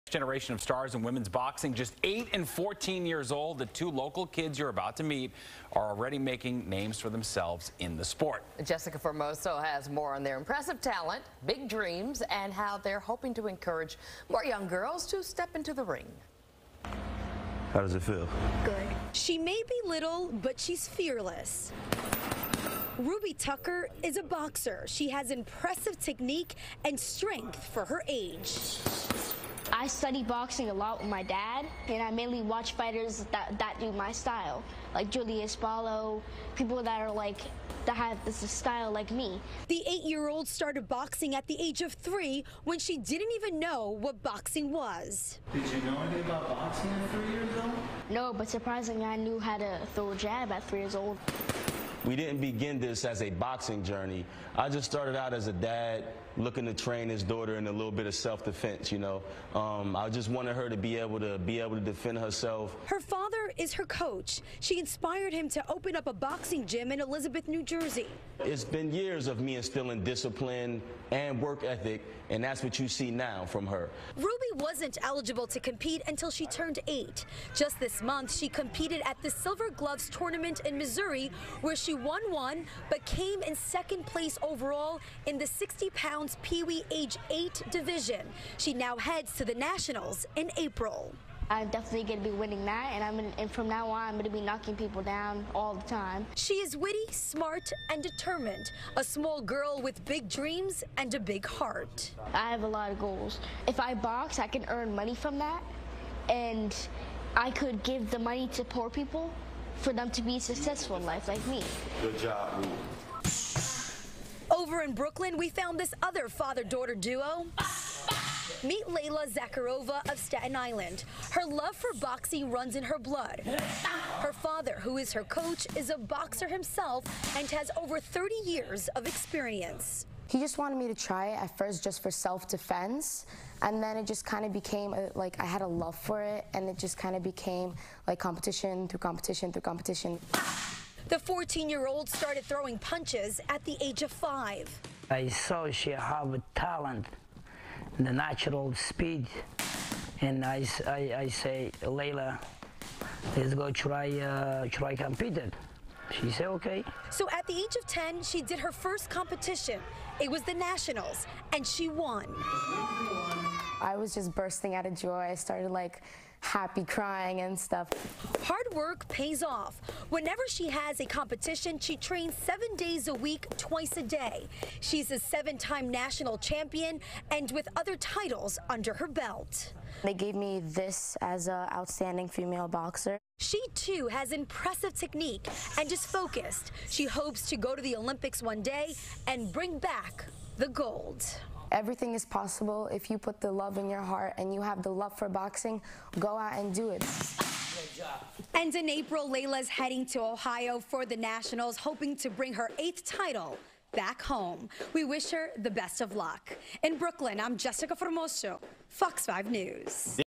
Next generation of female boxers
FOX 5 NY meets two local kids who are the next generation of stars in women's boxing.